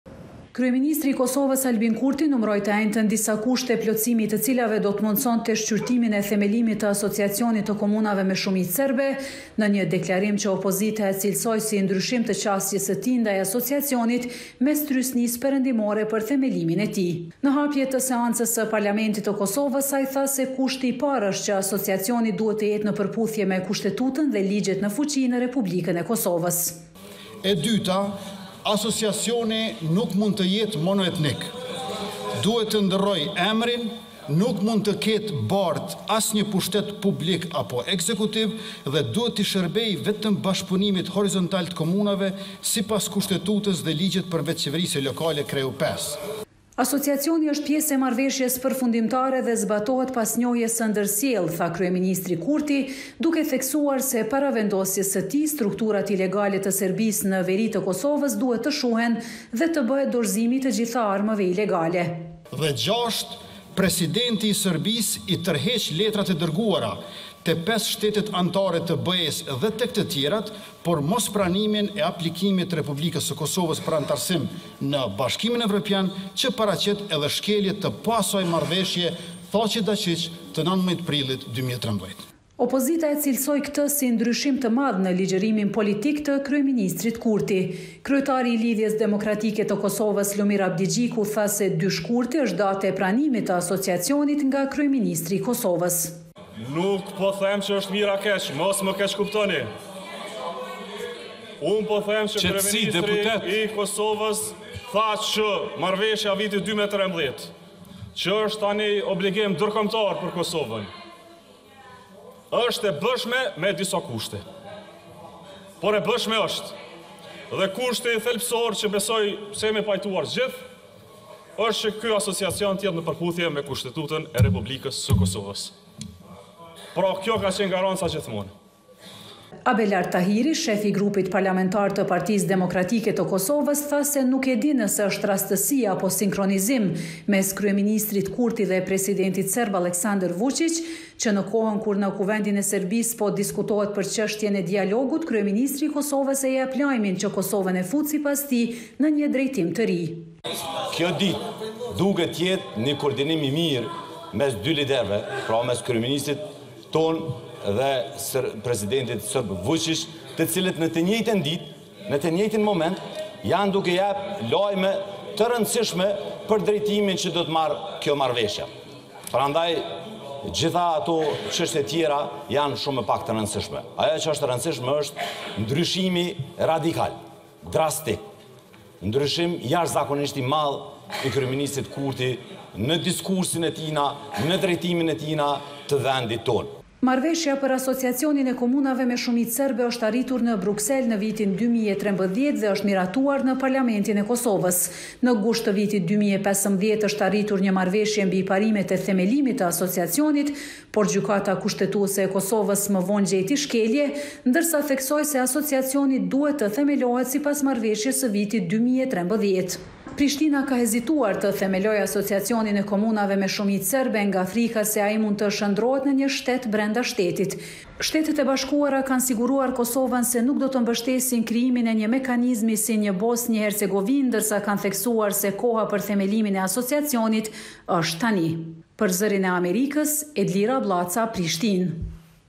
Kurti: Asociacioni i komunave me shumicë serbe – me kushte
Kryeministri i Kosovës, Albin Kurti, paraqiti të enjten gjashtë kushte të cilat tha se do të mundësonin shqyrtimin e themelimit të Asociacionit të komunave me shumicë serbe. Partitë opozitare e kritikuan atë gjatë punimeve të seancës së sotme në parlamentin e Kosovës, duke e cilësuar deklarimin si ndryshim të qasjes së tij të mëhershme mbi Asociacionin.